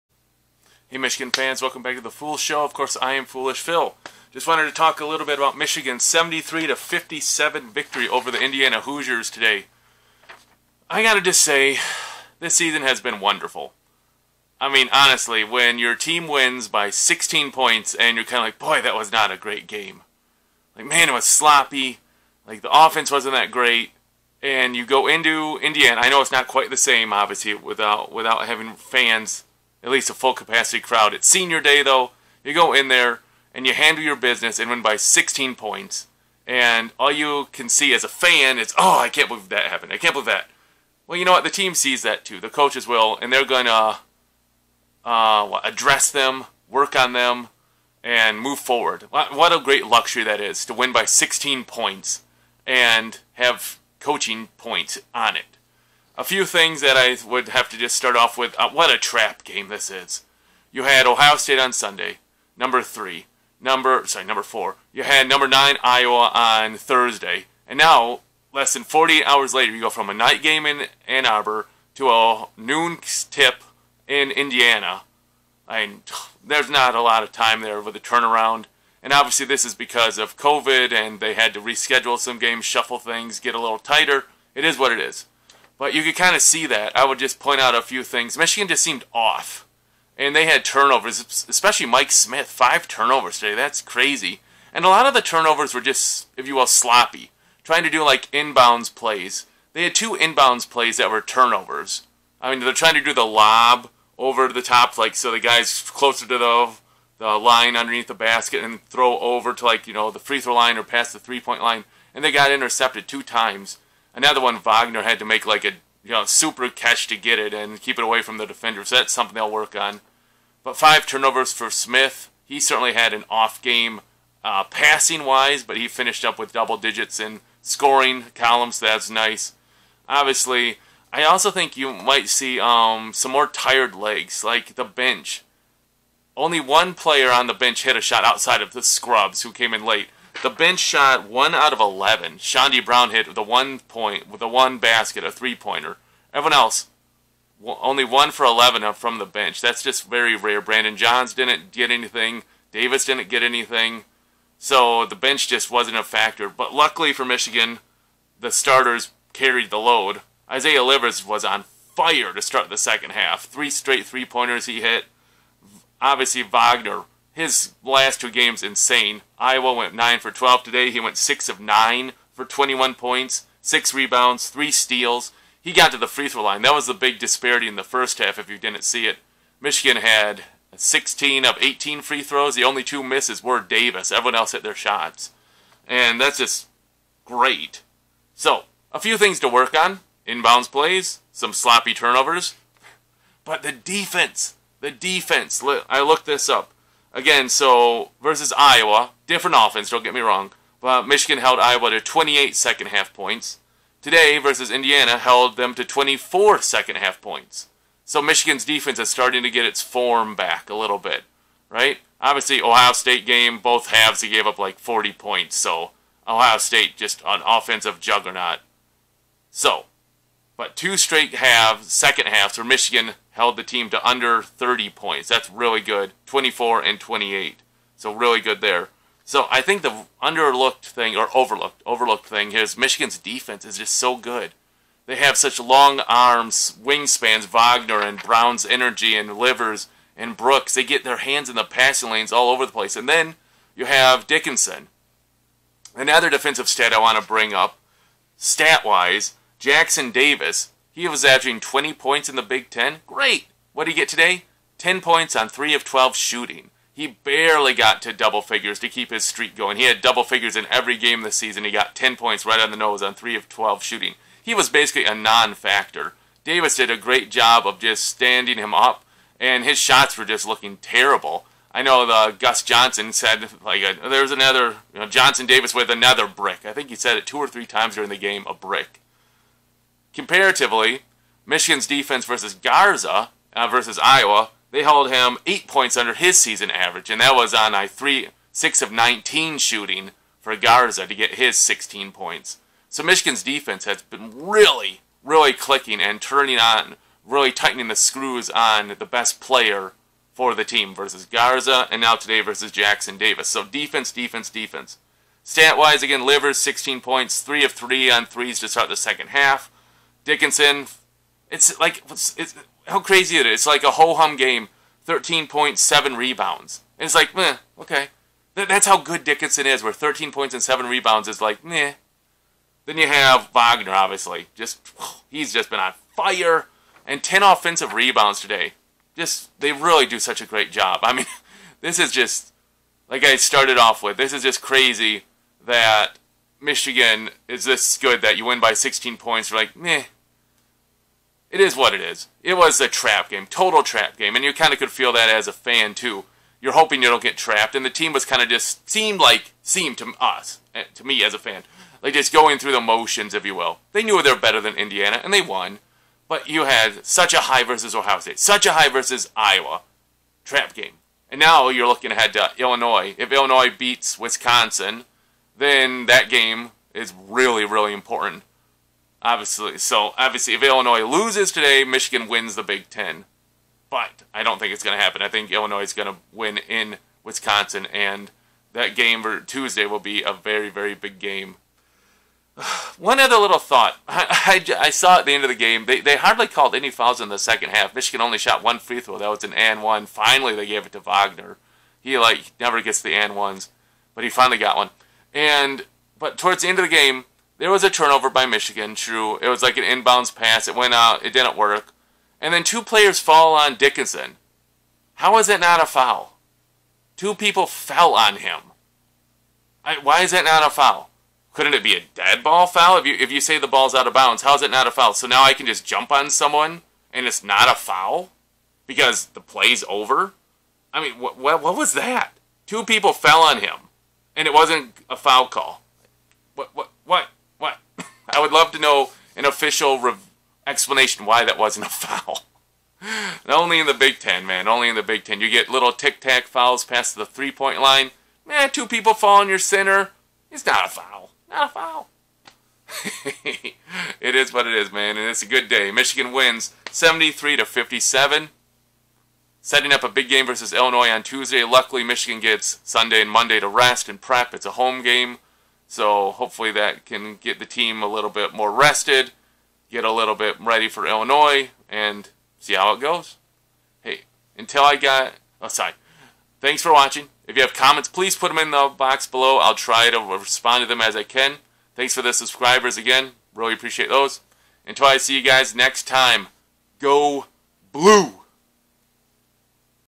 #3 Michigan handles Indiana and avoids the dreaded trap game on senior day
In a somewhat workmanlike effort the Wolverines build a 9 pt. halftime lead and cruised to a 16 point victory.  Michigan was led by Franz Wagner's 21 pts and Isaiah Livers 16.  The Michigan defense locked down on Indiana's Jackson-Davis, holding him to 10 pts on 3 of 12 shooting.  I think the Wolverines were a bit tired after an emotional week beating Ohio State and Iowa already this week.